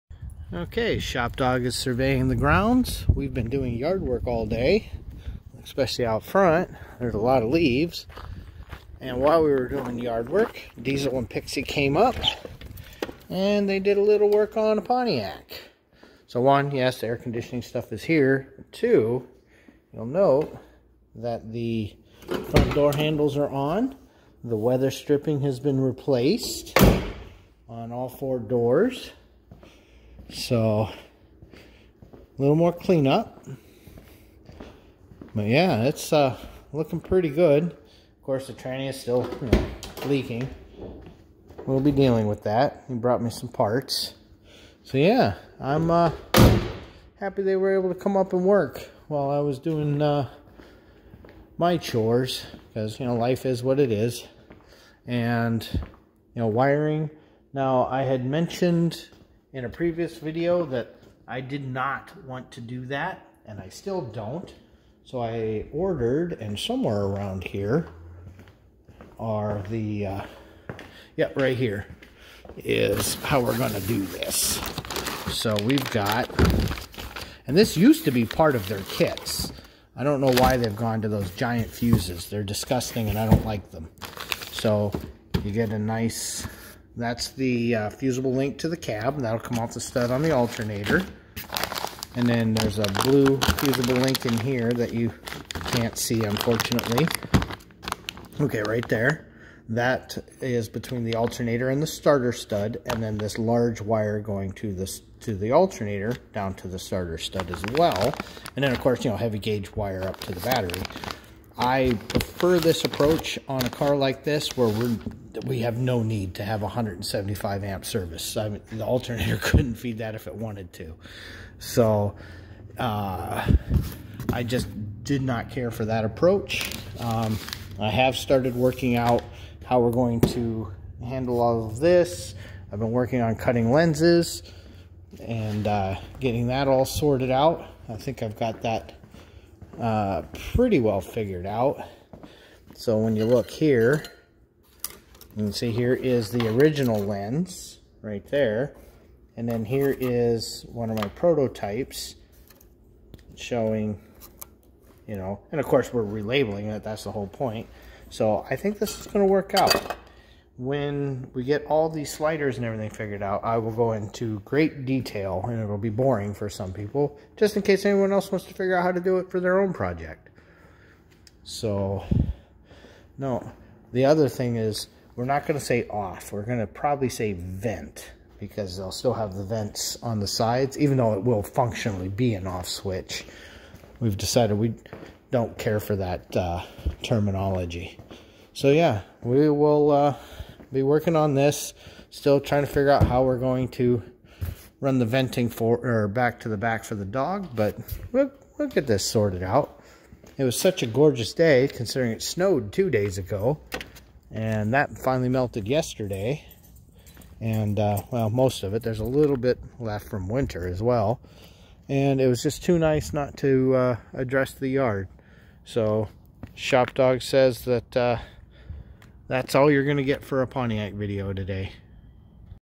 66 Pontiac Catalina Wagon part 10
Well, this is s strange one, I did not work on the car, but Diesel and Pixie came up and worked all day. We had yard work. Anyway, the doors now seal up, and the new weatherstripping is done!